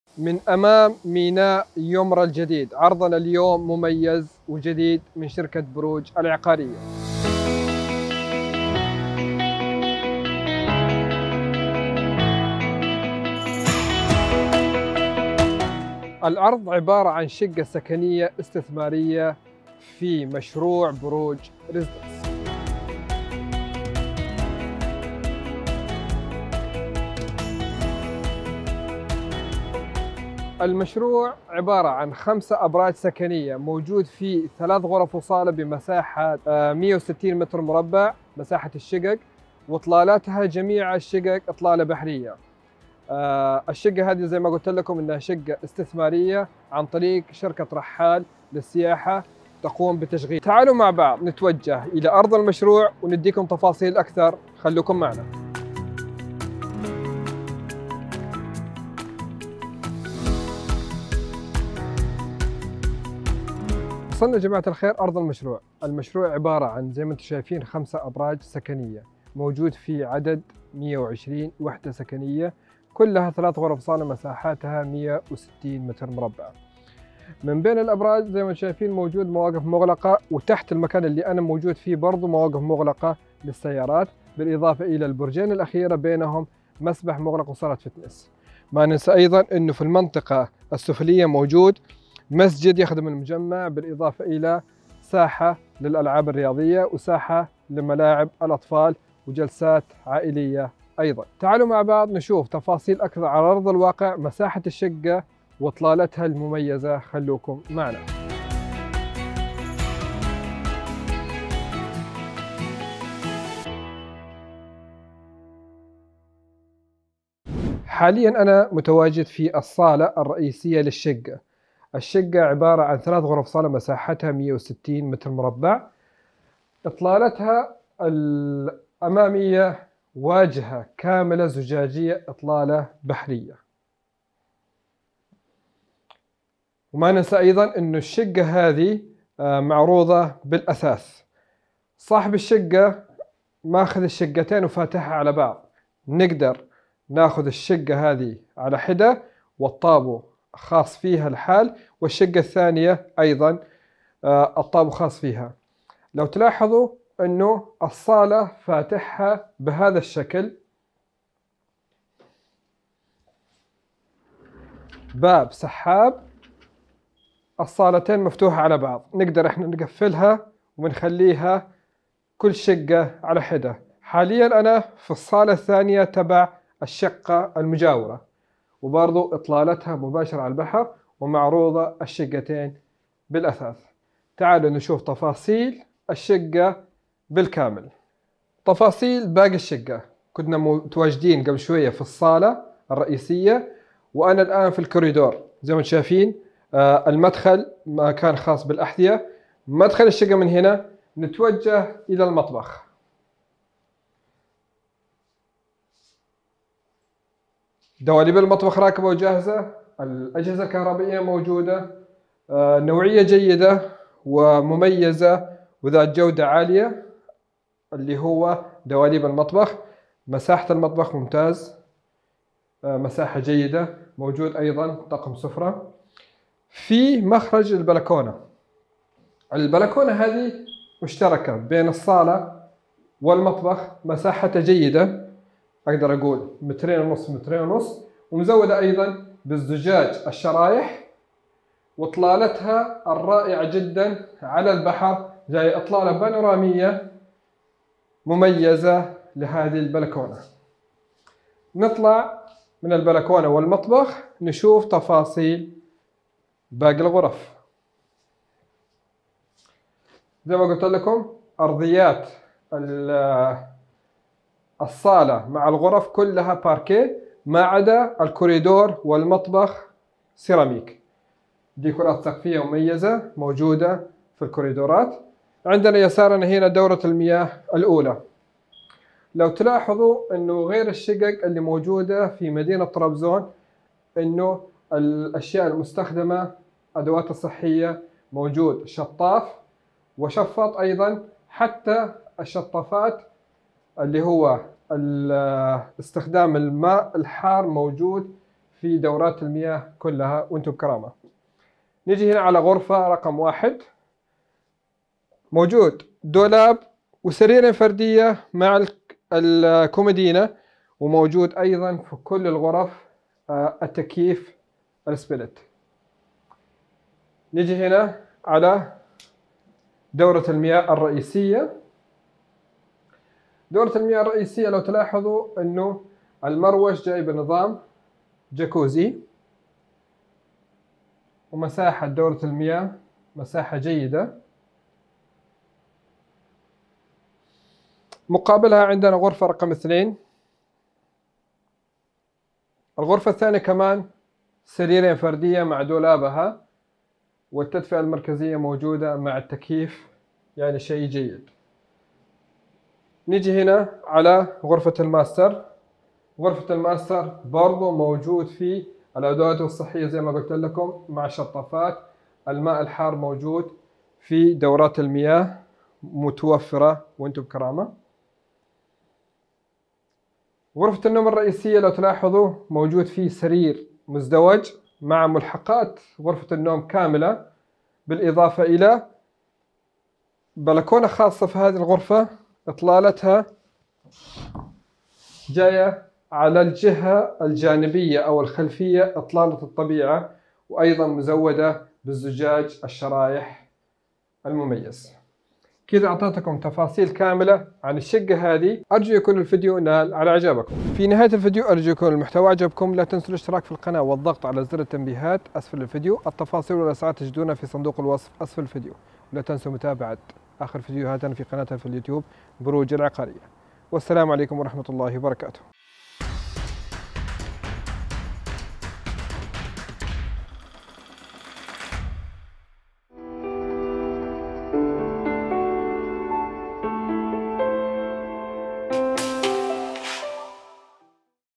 فرصة تملك شقق سكنية استثمارية 3 غرف وصالة ذات تشطيب مميز داخل مجمع سكني في منطقة يومرا الراقية 2022
فرصة تملك شقة سكنية استثمارية 3 غرف وصالة ذات تشطيب مميز داخل مجمع سكني في منطقة يومرا الراقية 2022

تفاصيل العقار كالتالي :
المدينة / طرابزون
المنطقة / يومرا
عدد الغرف / 3 غرف وصاله + 3 دورات مياه + مطبخ + بلكونتين
الدور / 9
المميزات /
- ما يميز المشروع بالدرجة الاولى اطلالتها السحرية على البحر والطبيعة .
- مسبح خاص ( مغلق ) للمجمع .
- صالة جيم ولياقة .
- مسجد خاص بالمجمع ( مساحتها ٦٠ متر مربع )
- مواقف سيارات مغلقة ( موقف لكل شقة )
- جلسات خارجية مطلة .
- ملعب كره سلة وقدم .
- العاب اطفال .
- مساحات خضراء .
- ممشى خاص بالمجمع .
- بوابه للمجمع وحراسه امنية وكاميرات مراقبة .
- مصعدين لكل عمارة .
- صالة اجتماعات .
- المجمع على ٤ شوارع .
- بجوار المجمع سوبر ماركت /ومطاعم / والمواصلات قريبة جدا/ جامع
- قريب من مرسى اليخوت الكائن في منطقة يومرا
-المسافه بين الموقع و جواهر مول (3 دقائق)
المسافه بين الموقع والمطار ( 7 دقائق )
المسافة بين الموقع وميدان طرابزون (10 دقائق)
-قريب ايضا من شاطئ السباحة خلف فندق نوفوتيل

مميزات الشقق /
تتميز الشقق في مشروعنا بالتالي :
- واجهة زجاجية للصاله ( اطلالة بانورامية ) .
- جاكوزي .
المطبخ جاهز بالكامل
الشقة مؤثثه بالكامل
- امكانية استثمارها من خلال شركة رحال السياحية بعوائد تصل الى 10%

رابط الواتساب المباشر للتحدث معي